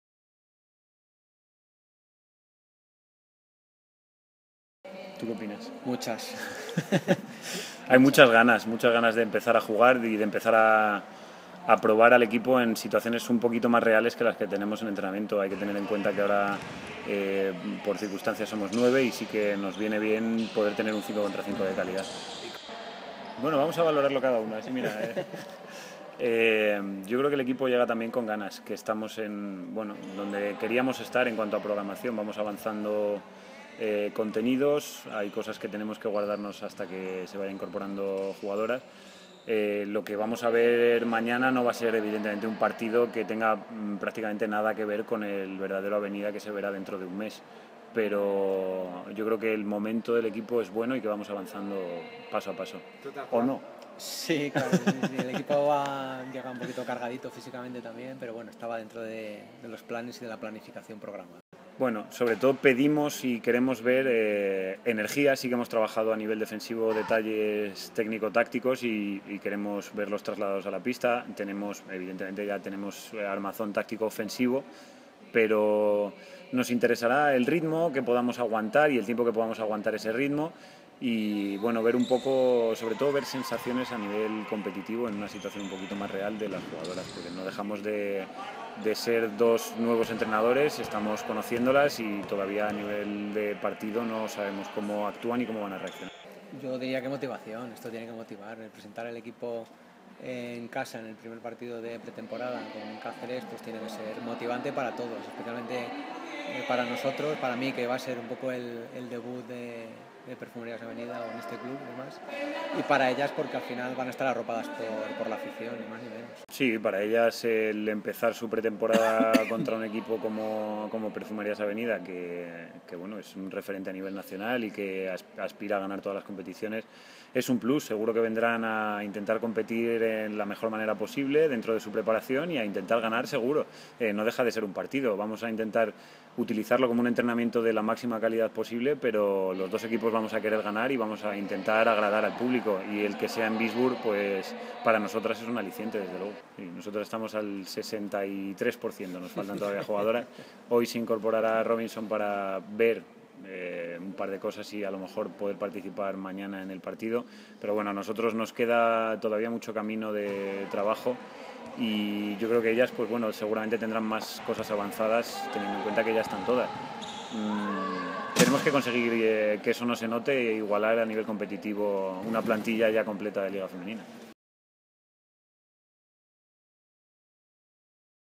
Previa Torneo Ferias 14
Nuestro cuerpo técnico formado por Alberto Miranda y Avelino García analizan el primer partido de la pretemporada, el Trofeo de Ferias ante Aql-Qazere